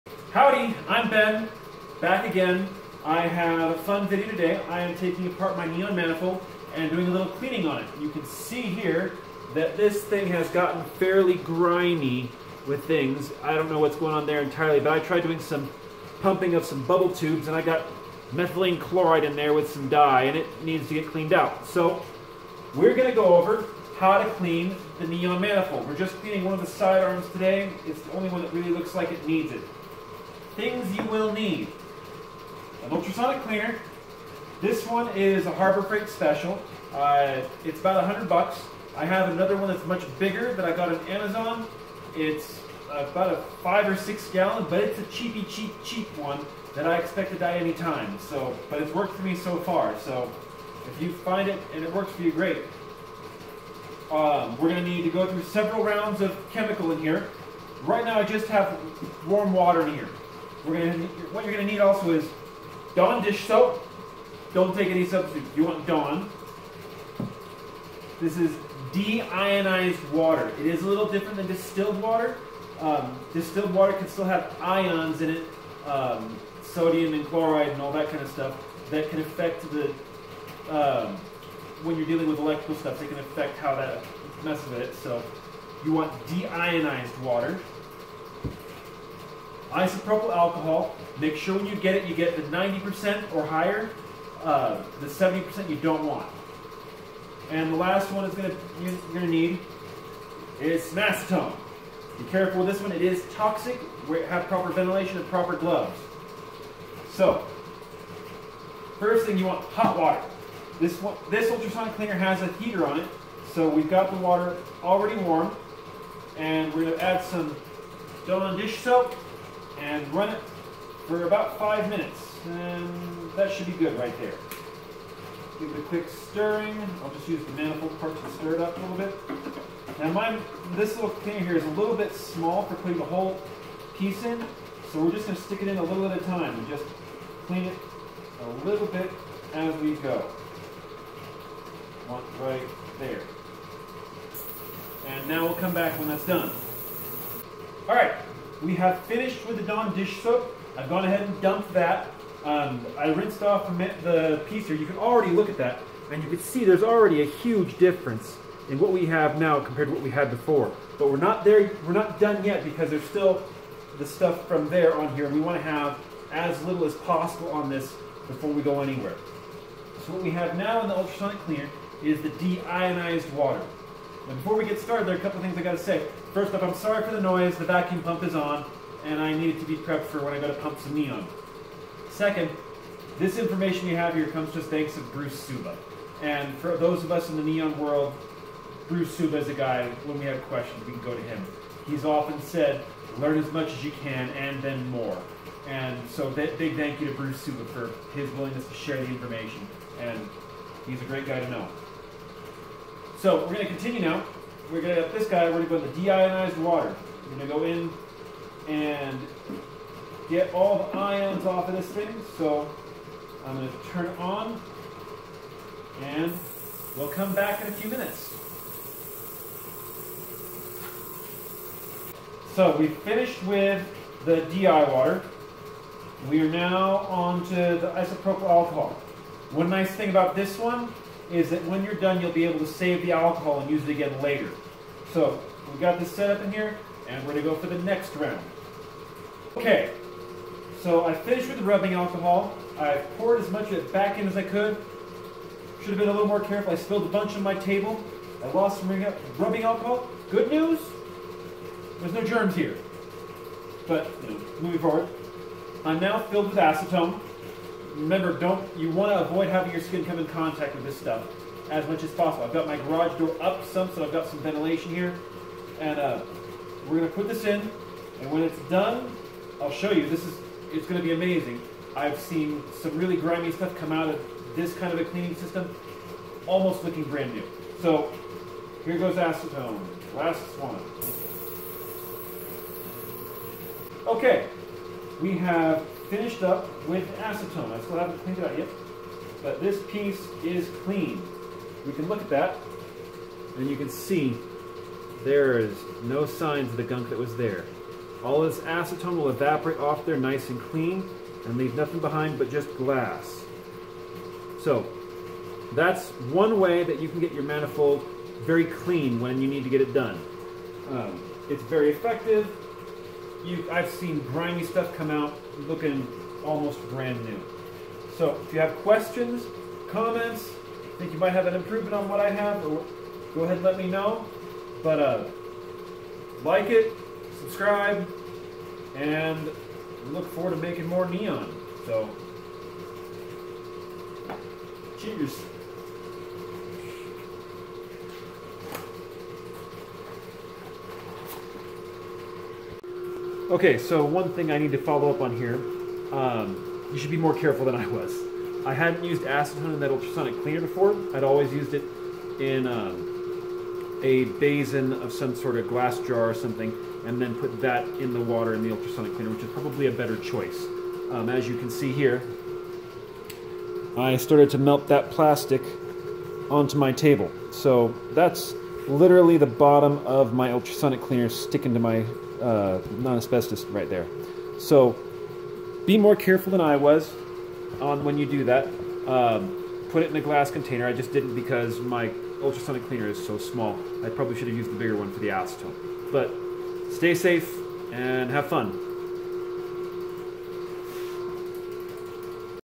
Cleaning a Glass Neon Manifold
Steps to clean your manifold. It will work on some of the grimiest stuff off and bring the glass back to looking new. It is also a method for cleaning glass for use in crackle tubes and other plasma applications.